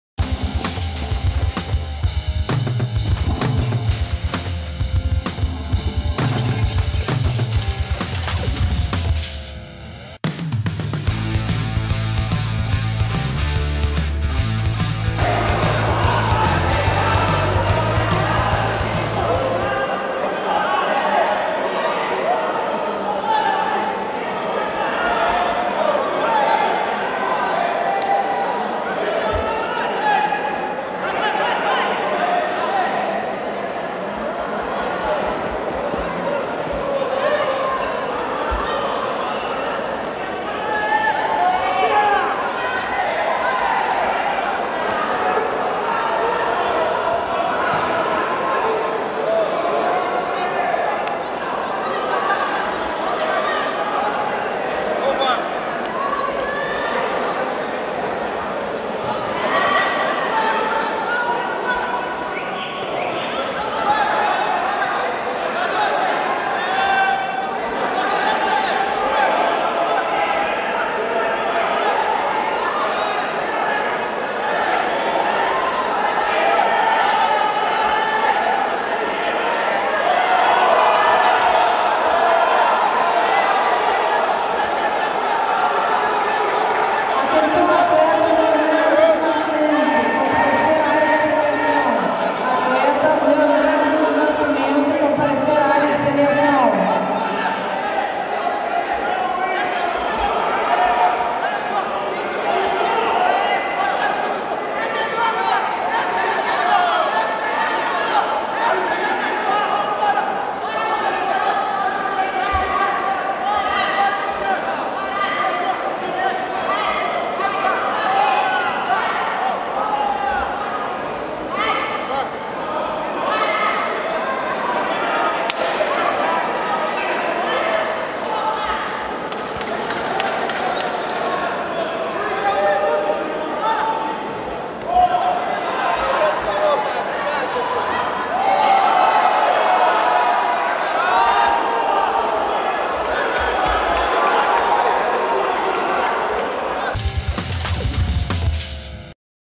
SULAMERICANO CBLP 2015 - Luciano Matos x Luiz Fernando
CAMPEONATO SULAMERICANO DE JIU-JITSU PROFISSIONAL 2015 - CBLP
25 de Janeiro de 2015 - Niterói - Rio de Janeiro
Ginásio do Canto do Rio